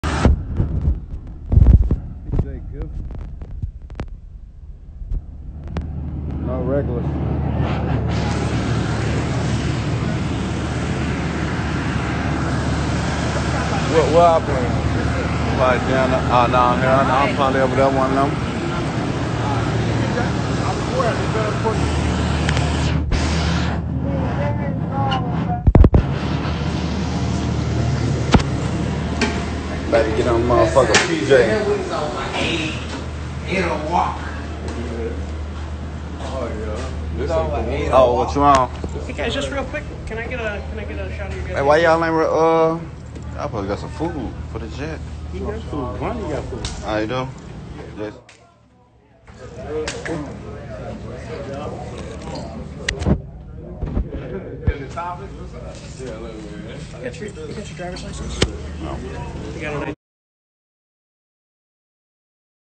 Muwop get asked for ID 🤪. He doesn’t have it.
Is Muwop traveling with Durk to rolling loud festival??